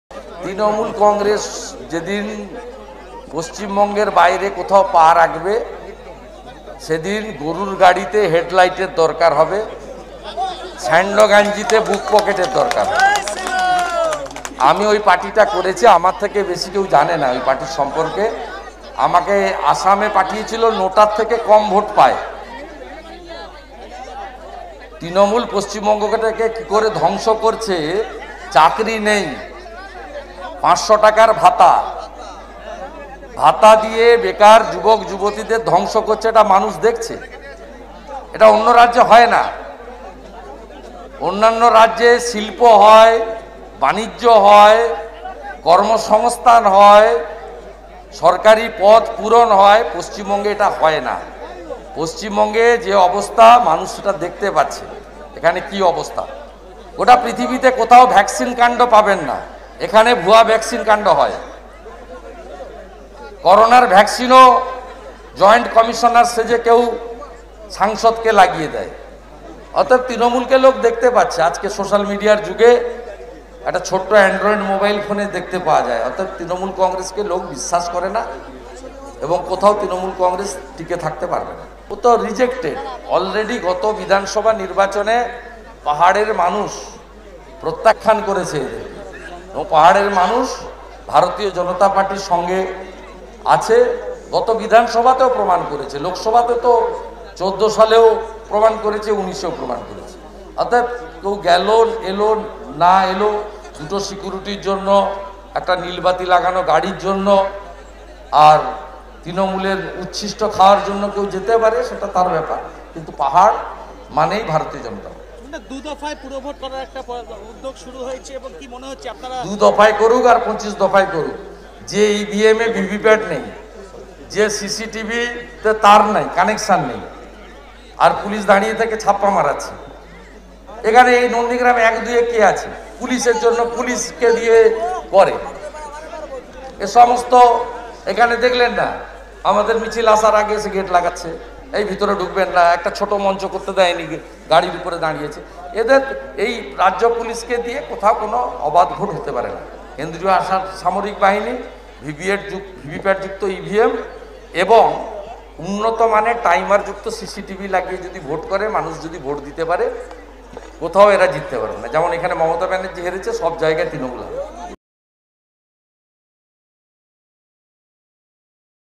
তৃণমূল রাজ্যের বাইরে নোটার থেকে কম ভোট পায় : শুভেন্দু অধিকারী |oneindia bengali
তৃণমূল রাজ্যের বাইরে নোটার থেকে কম ভোট পায় : শুভেন্দু অধিকারী